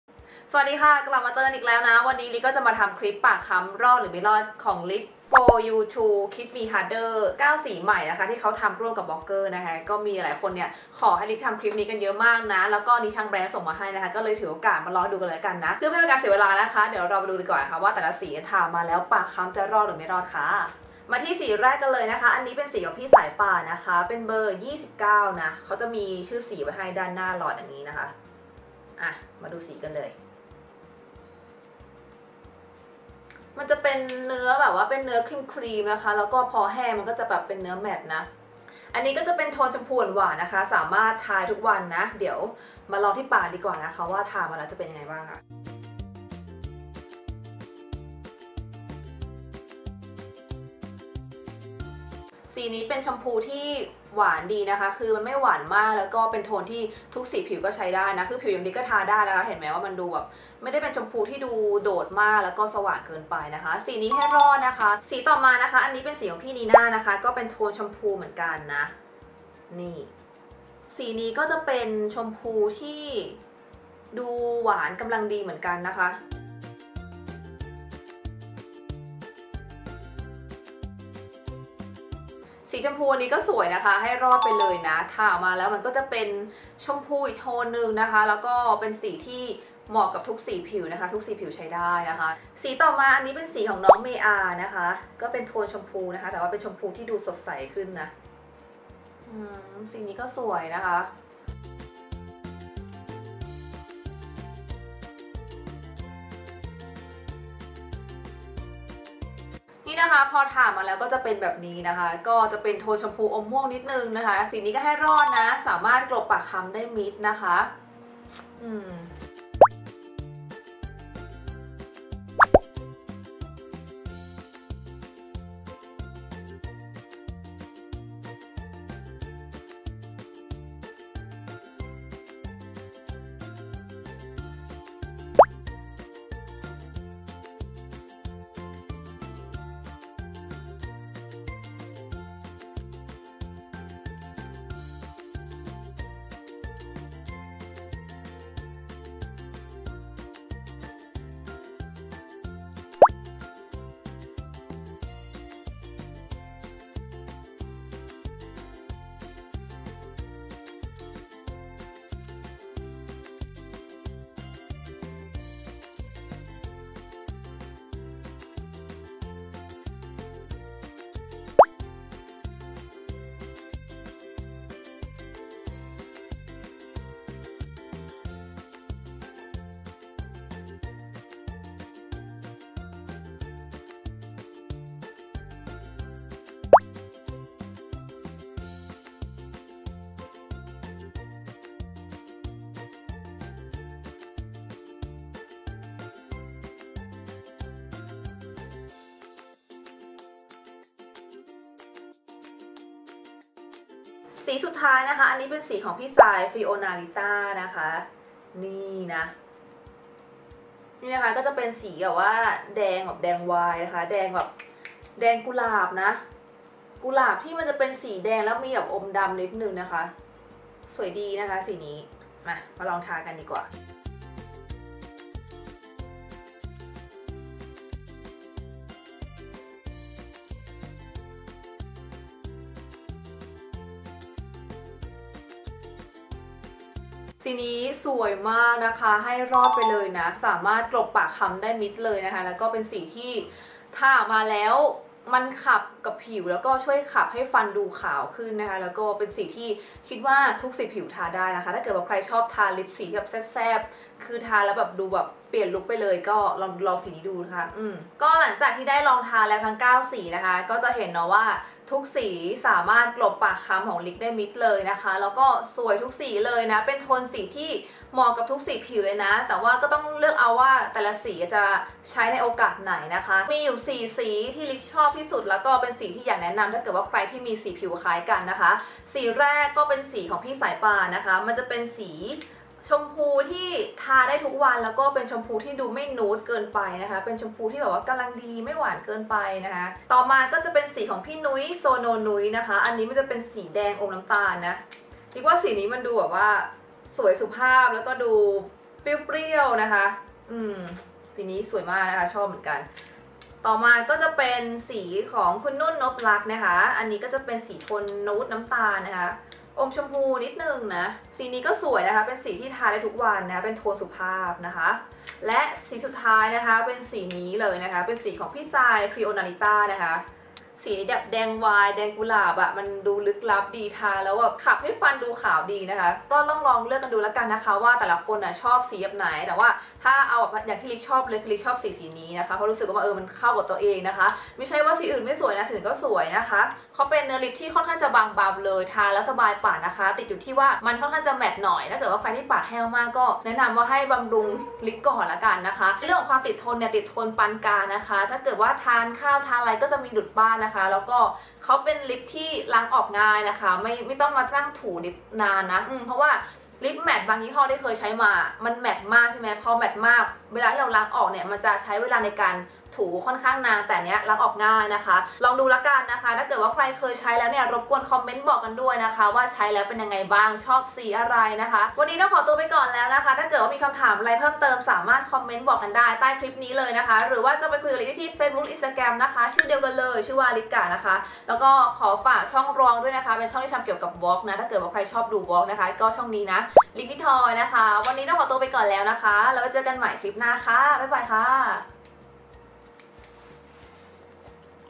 ปากคล้ำ รอด หรือ ไม่รอด : ลิป 4U2xBlogger ทั้งหมด 9สี | Licktga
กดตรงนี้เพื่ออ่านข้อมูลเพิ่มเติม ↴
✎ คลิปนี้ทำขึ้นมาเพื่อสาวๆที่มีปากคล้ำเหมือนกัน ให้ตัดสินใจง่ายขึ้น ว่าควรซื้อมาลองดีไหม ทั้งนี้ขึ้นอยู่กับการติดสินใจของเพื่อนๆด้วยนะจ้ะ

อ่านแบบภาพ -

กล้อง - Canon eosm3
โปรแกรมตัดต่อ - Sony vegas pro14
คอนแทคเลนส์ - Neo cosmo สี shimmer brown
ลิป - 4U2xBlogger